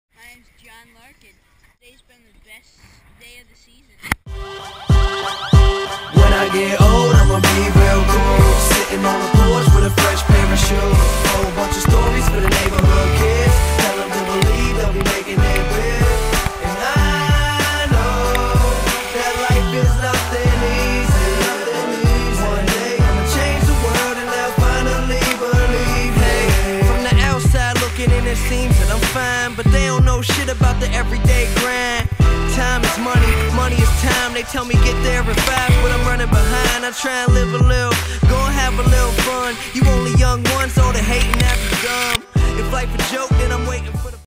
Jonny Larkin
Last few days of jonny throwing down